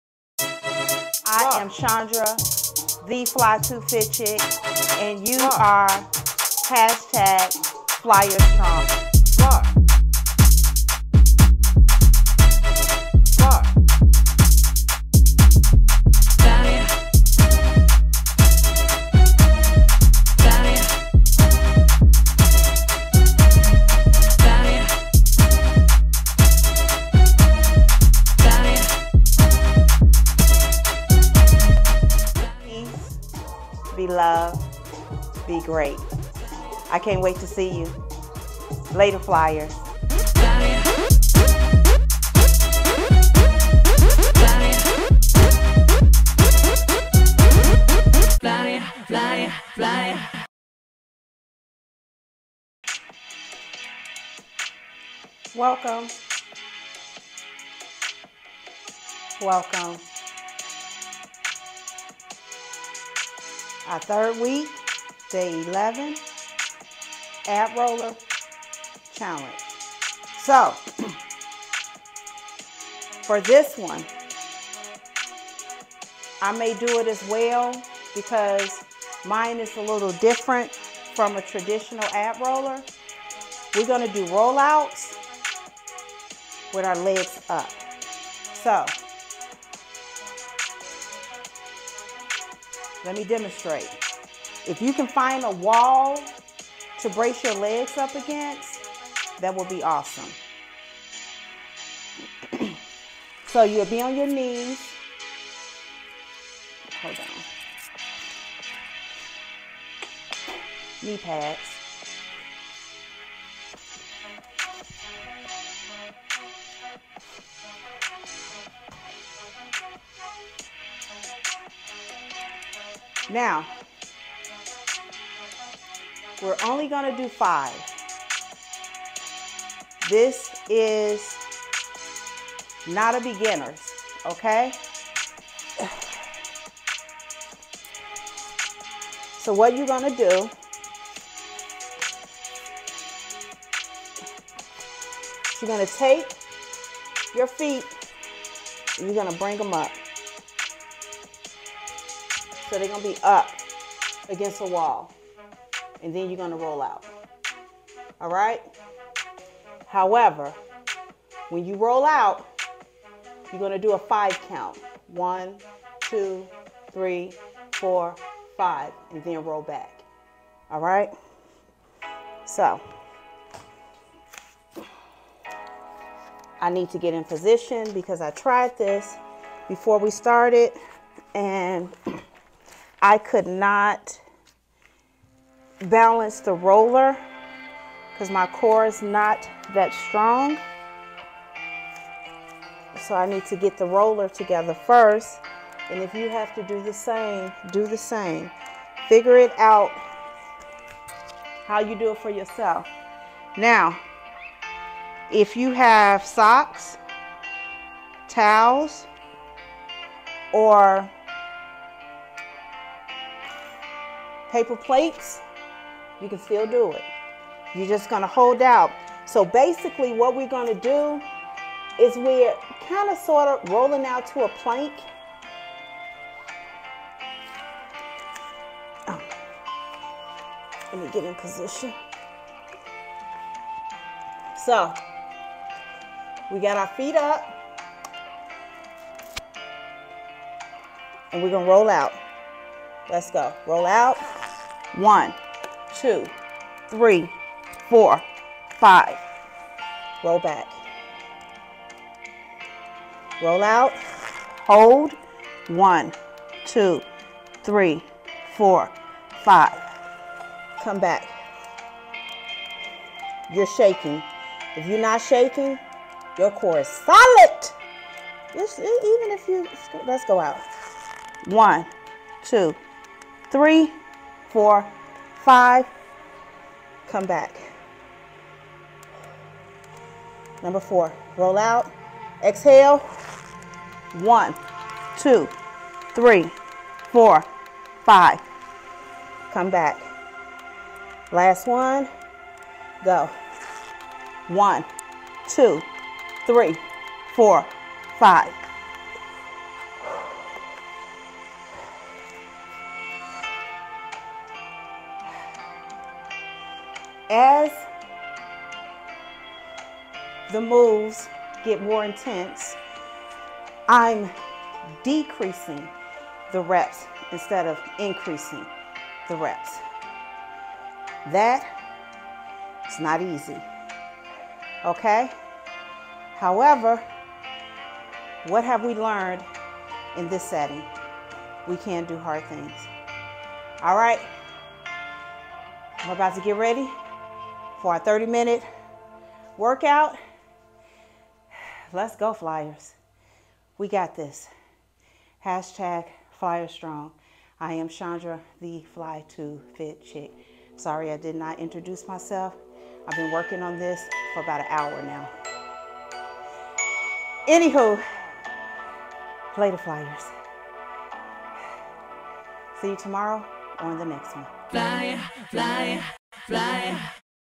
Epic 20-Day Ab Roller Challenge for a STRONGER CORE - Day 11
Join us on this epic 20-day Ab Roller Challenge to strengthen your core and see incredible results! Follow along as we guide you through each day of the challenge, helping you build a stronger and more defined midsection. Get ready to push your limits, improve your stability, and sculpt your abs with this intense workout program. Are you up for the challenge? Let's do this!